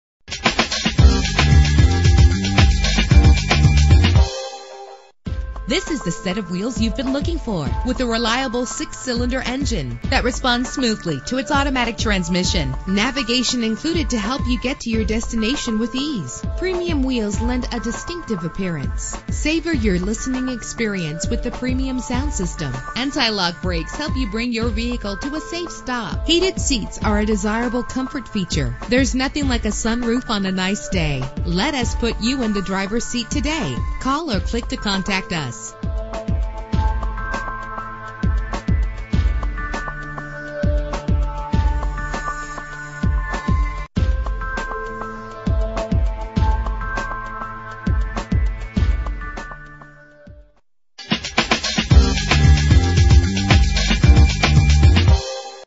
2012 Chrysler 200 - Gainesville GA
Year: 2012
Make: Chrysler
Model: 200
Trim: S
Engine: 3.6 liter 6 cylinder 24 valve
Transmission: Automatic
Color: Bright Silver Metallic
Mileage: 0
Address:
3115 Frontage Rd